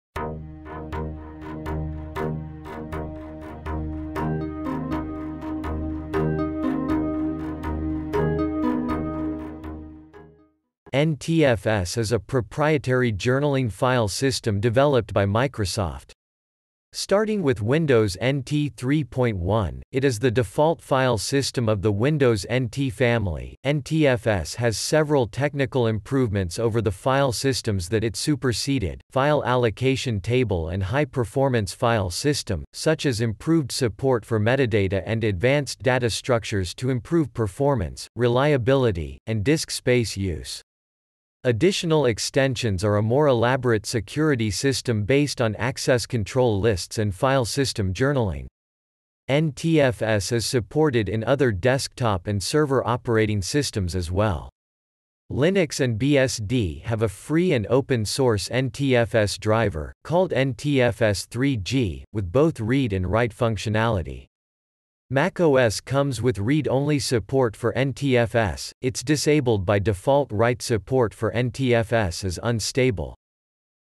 What is NTFS? Explain NTFS, Define NTFS, Meaning of NTFS
~~~ NTFS ~~~

Title: What is NTFS? Explain NTFS, Define NTFS, Meaning of NTFS

Description: NTFS is a proprietary journaling file system developed by Microsoft. Starting with Windows NT 3.1, it is the default file system of the Windows NT family.NTFS has several technical improvements over the file systems that it superseded – File Allocation Table and High Performance File System – such as improved support for metadata and advanced data structures to improve performance, reliability, and disk space use. Additional extensions are a more elaborate security system based on access control lists and file system journaling. NTFS is supported in other desktop and server operating systems as well. Linux and BSD have a free and open-source NTFS driver, called NTFS-3G, with both read and write functionality. macOS comes with read-only support for NTFS; its disabled-by-default write support for NTFS is unstable.